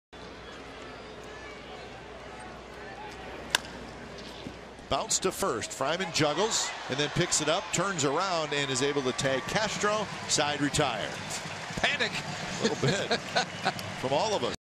HOU@OAK: Freiman recovers to nab Castro at first
7/22/14: Nate Freiman fields a slow grounder along the first-base line, stumbles then recovers to tag out Jason Castro in the baseline